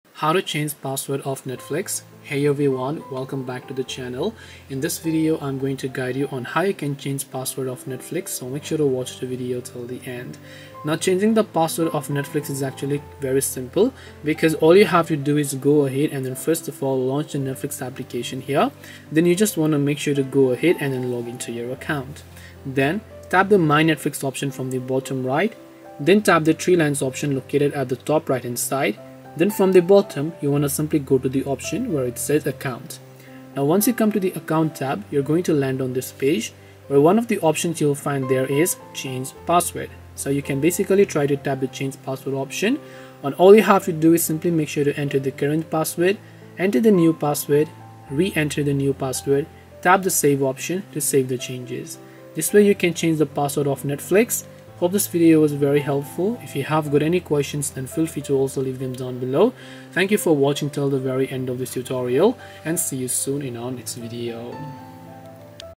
How to Change Password of Netflix
In this tutorial video, I will quickly guide you on how you can change the password of Netflix. So, make sure to watch this video till the end. We try to be responsive to as many questions as possible.

► Chapters:
0:00 Introduction
0:11 Change Password
0:52 Conclusion